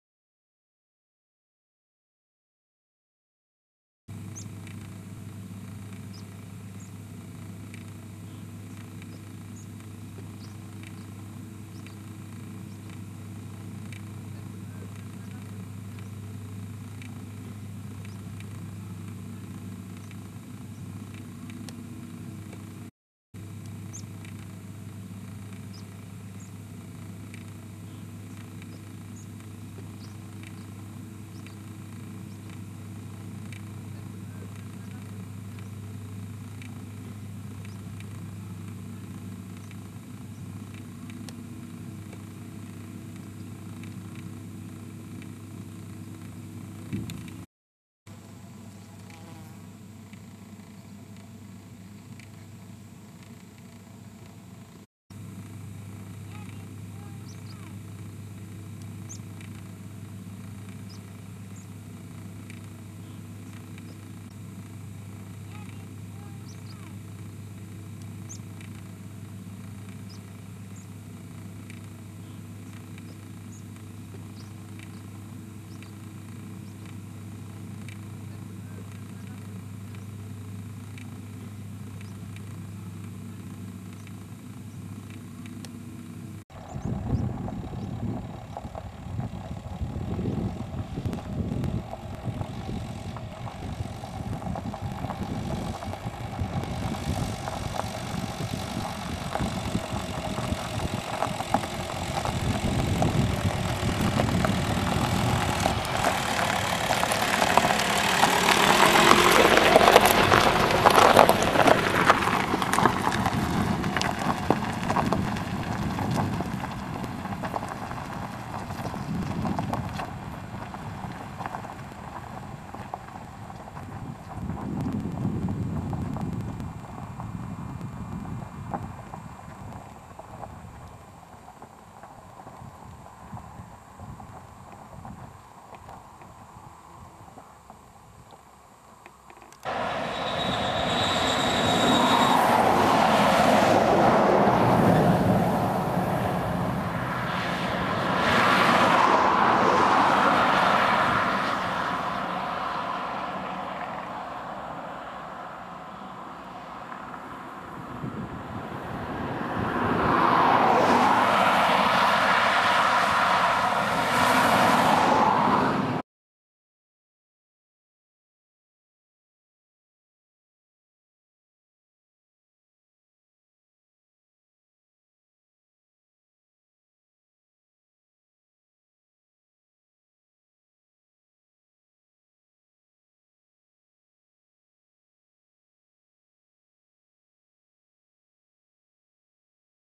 Travelling in Scotland (UK)
Touring Scotland in a VW T5 Transporter and tent. Have a look at the best stretch of the A68, one of the UK's most "adventurous" major roads, with numerous hills and Blind Summits back into Northern England.